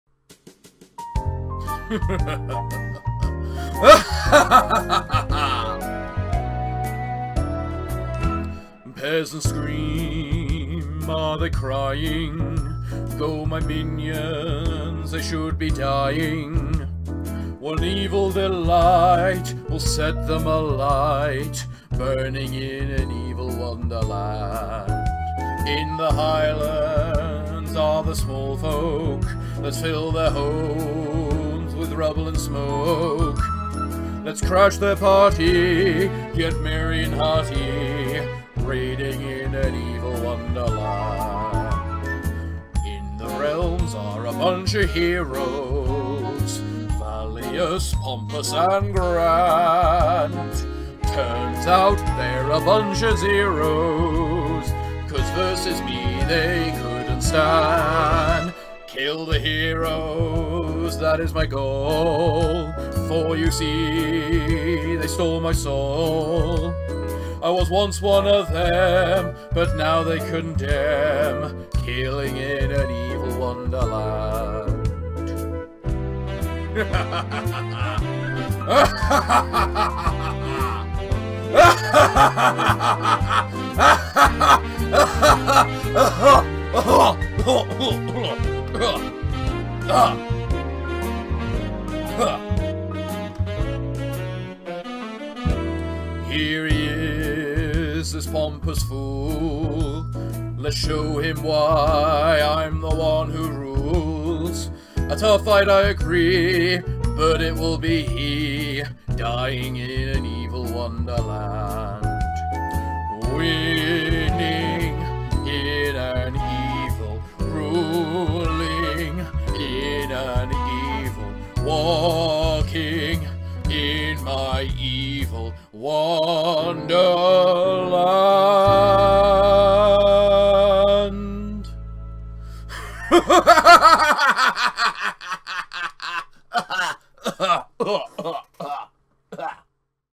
A Gentlemanly Christmas: Walking in an evil wonderland
Oh what good times we had playing this game. Oh what good times we'll have with the next. But that will come at a later date. For now let's just take a stroll through our evil wonderland!

And always remember. In the world of gentlemen the top hat is the crown.

All gentlemen and gentleladies are more than welcome. Come to my manor and have some tea!

Do you wish to support me and my content? Look no further for you are able to noble soul. Simply head here: